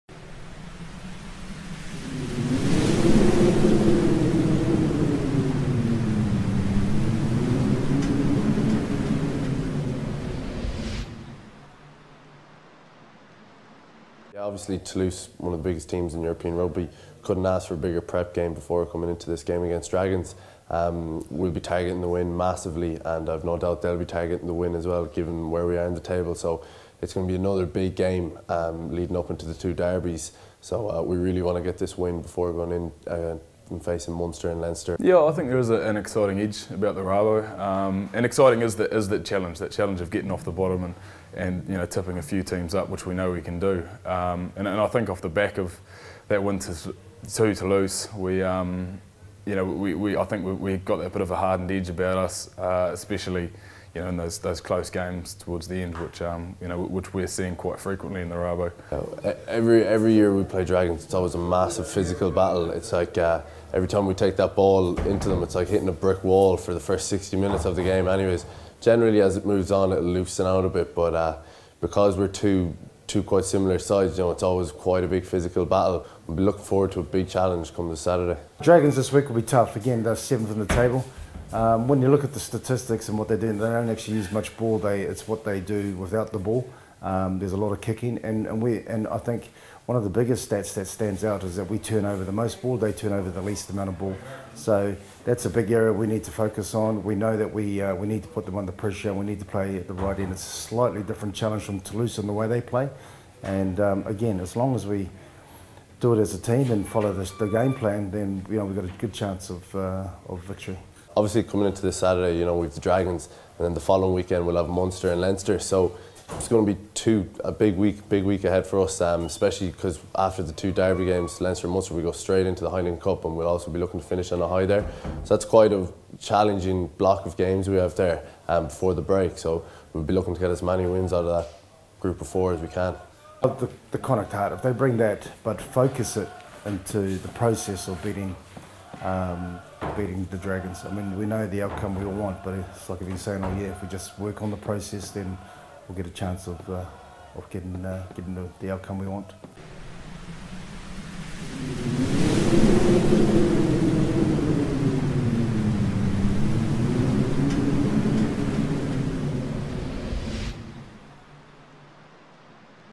Pat Lam & players preview Dragons
Connacht Head Coach Pat Lam and players Eoin McKeon & Jake Heenan look ahead to the visit of the Dragons in round 10 of the RaboDirect  PRO12.

Connacht v Dragons
Sat 21st Dec
KO 5pm